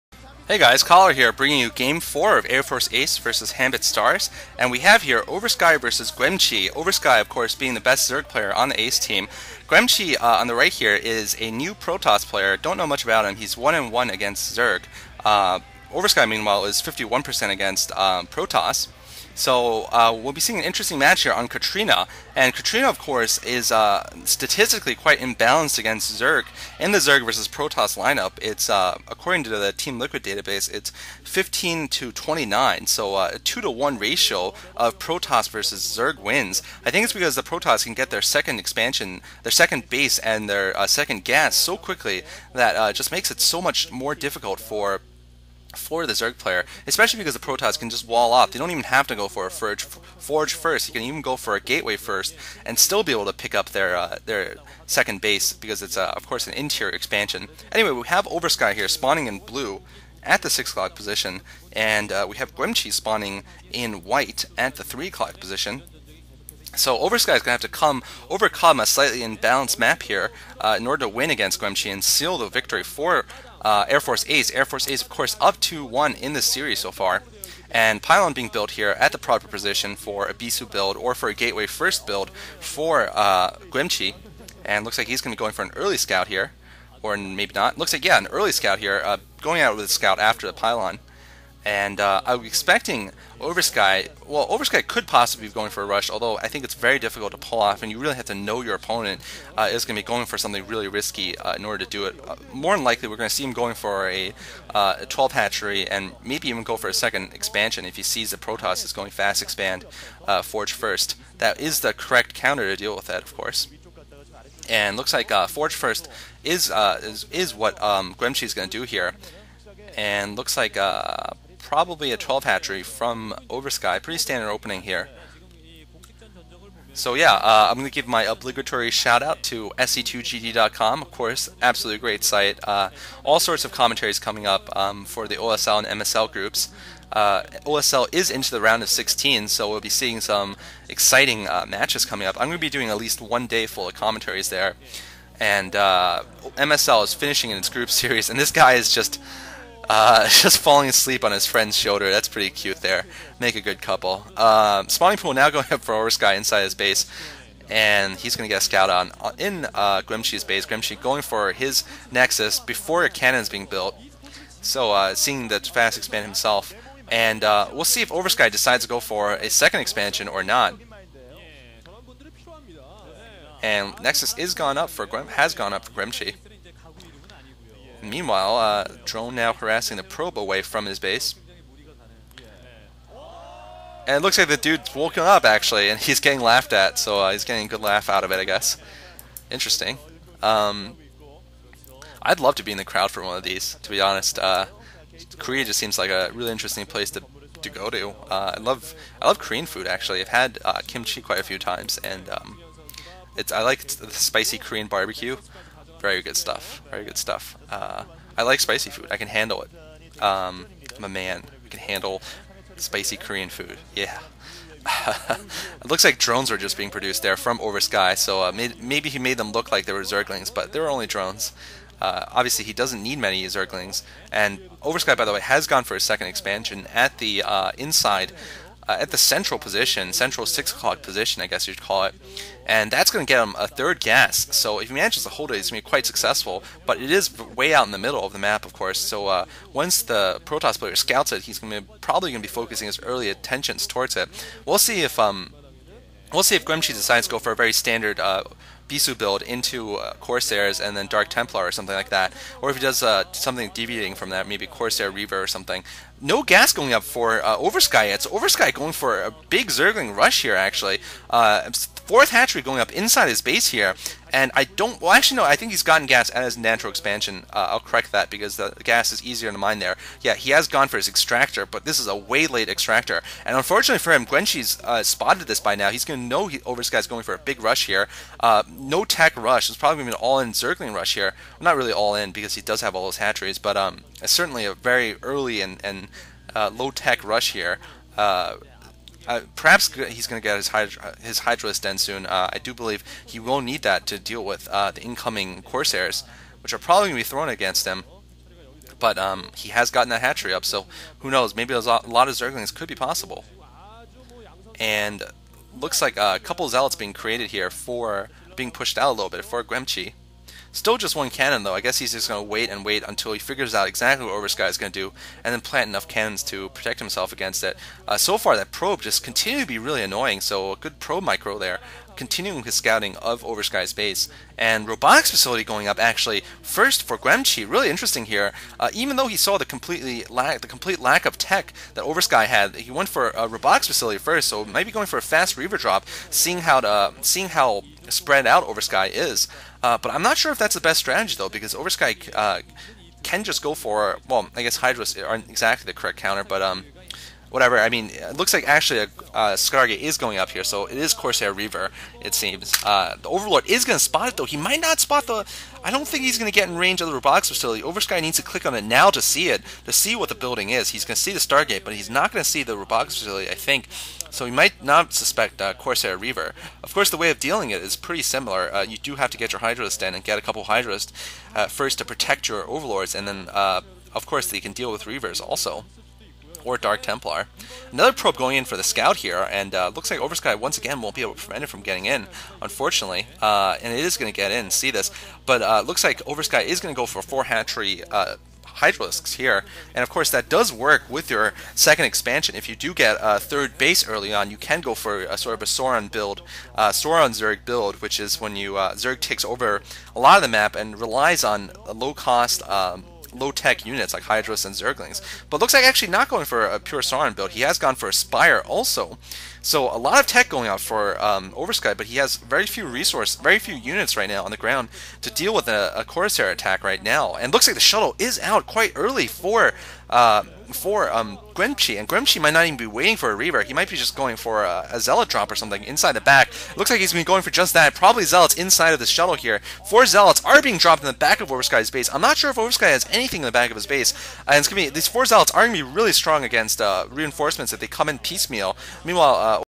ACE v Hanbit Set 4 (P1/2) Oversky v Guemchi
Hey all,

Underdog Air Force ACE versus plain-jane Hanbit Stars here in this Proleague series played on 5/10/08. Ace is pushing its strongest lineup I've seen in a while, with Daezang, Boxer, Oversky, and the new 2v2 team of MuMyung/Foru in their first four game lineup.  Boxer is going to play TvZ!

Set 4
Oversky [Ace] v Guemchi [Hanbit]
Oversky: Blue Z, 6
Guemchi: White P, 3

Oversky: 51% ZvP.  Guemchi: 50% PvZ.

Map: Katrina SE